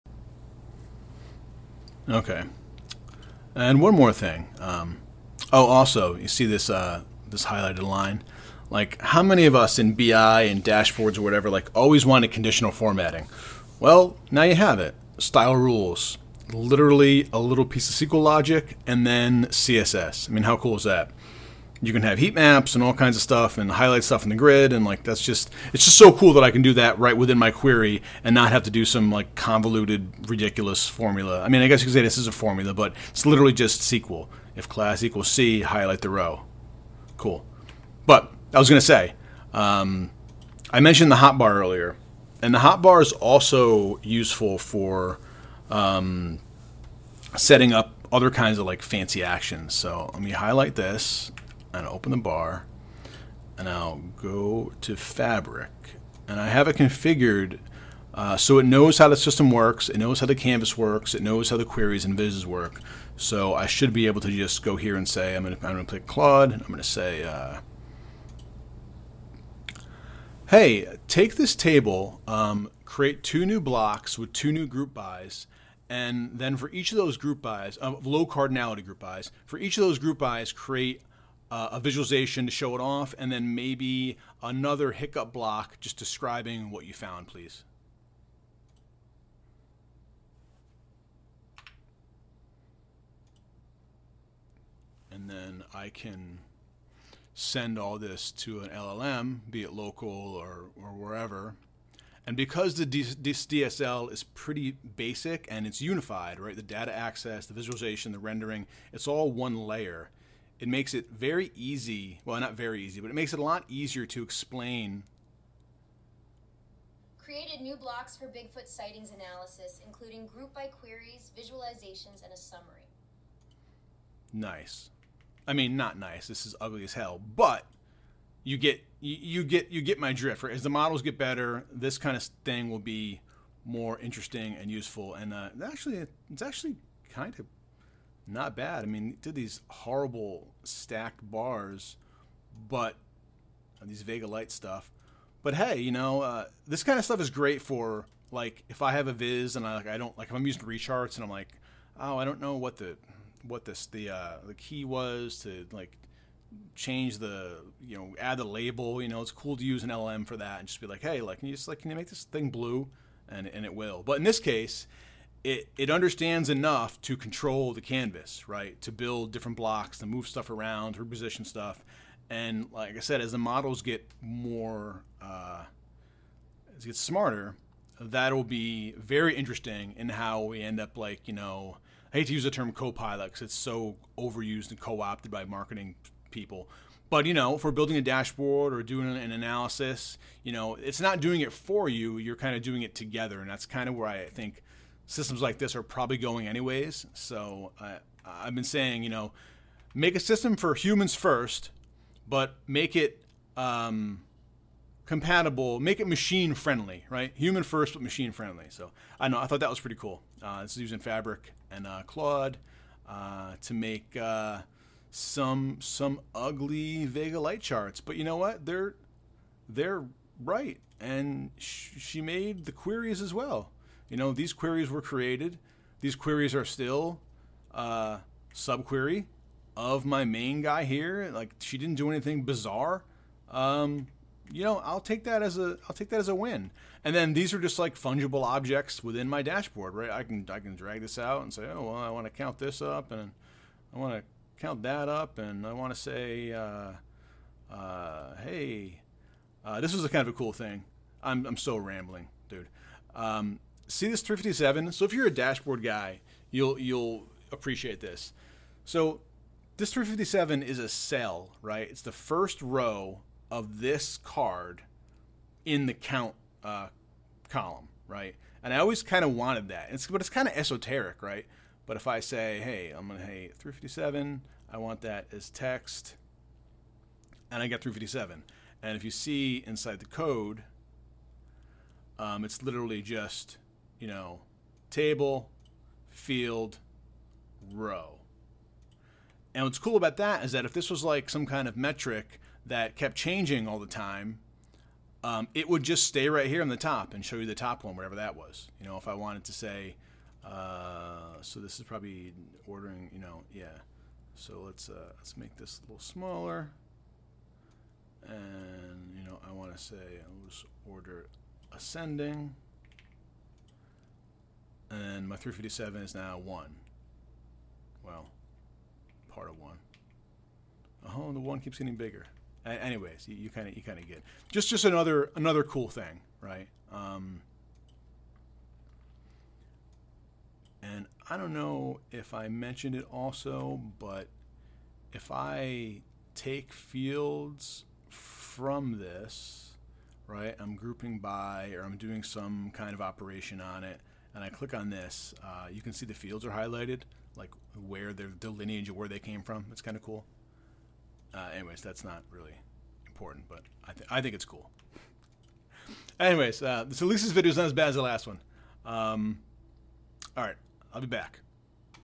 RVBBIT 0.1.0 Alpha - AI Canvas feedback loop
Overview of RVBBIT's using an LLM to mutate the canvas - Alpha avail now